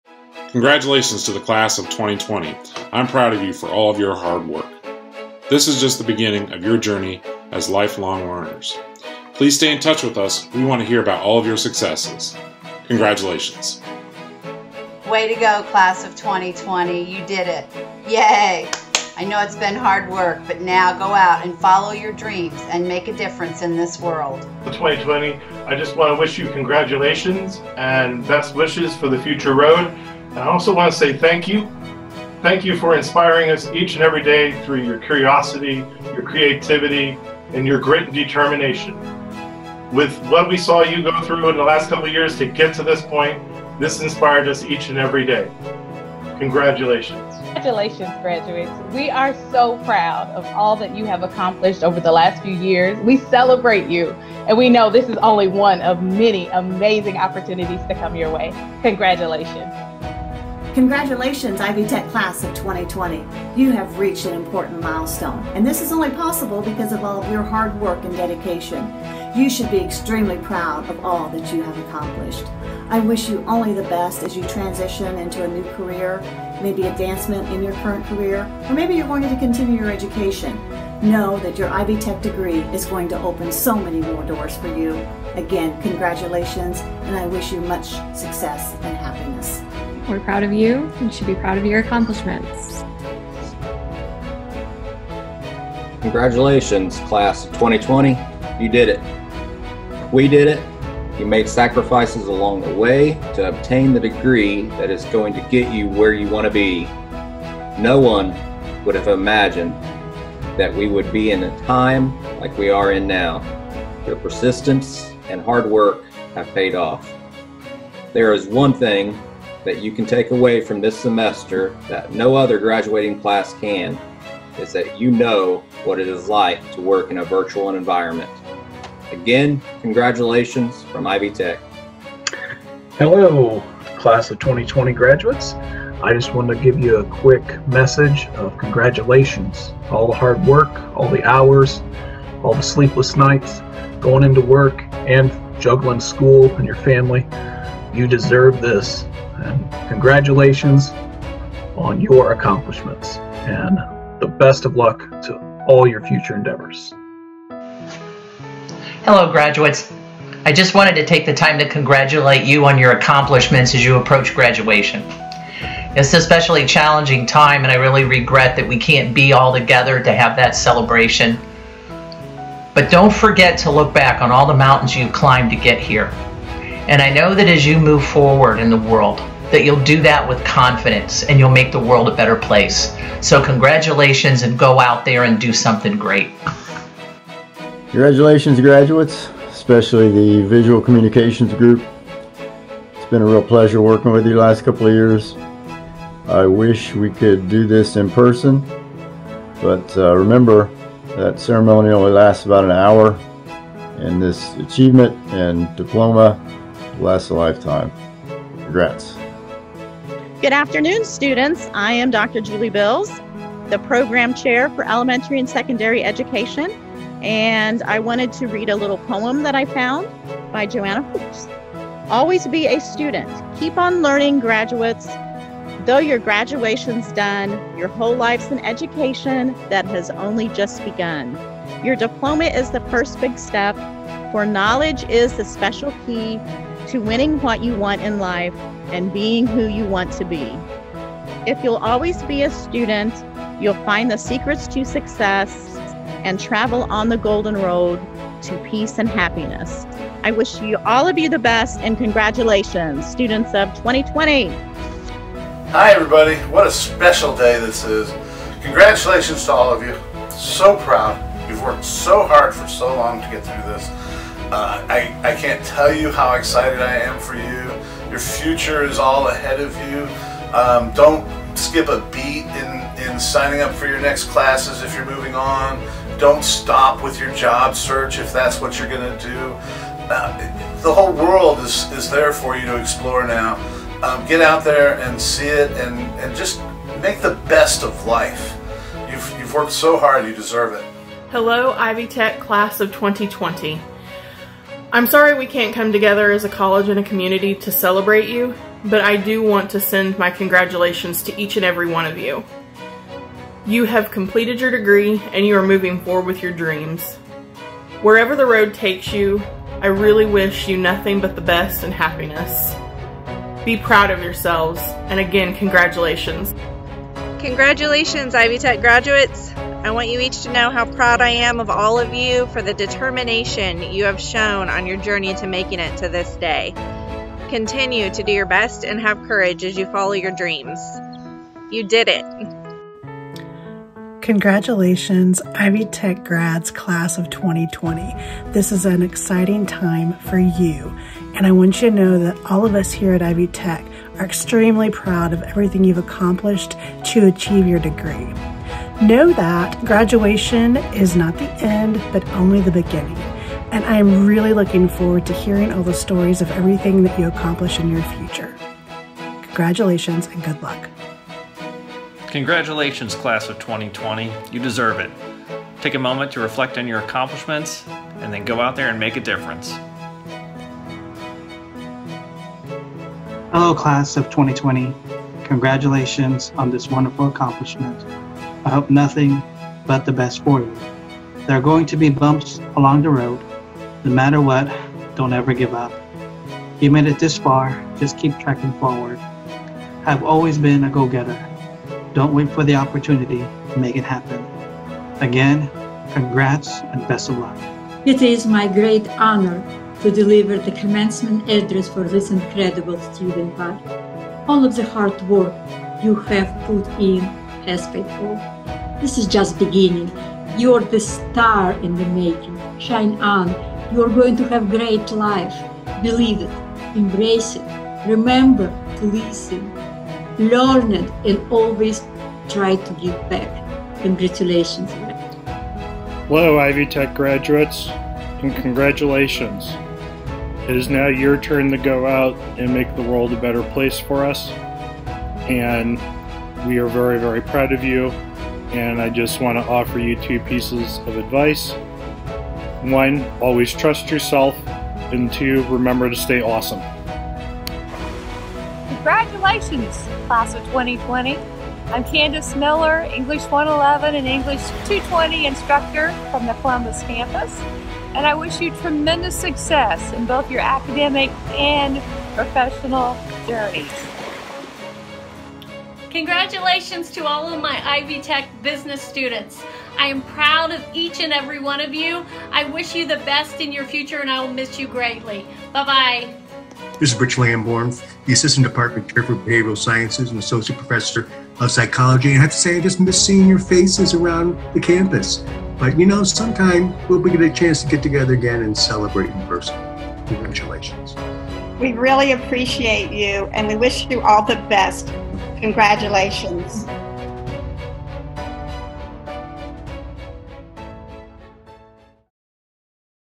To the Class of 2020!
Faculty members of Ivy Tech Community College Columbus offer virtual congratulations to the graduating class of 2020.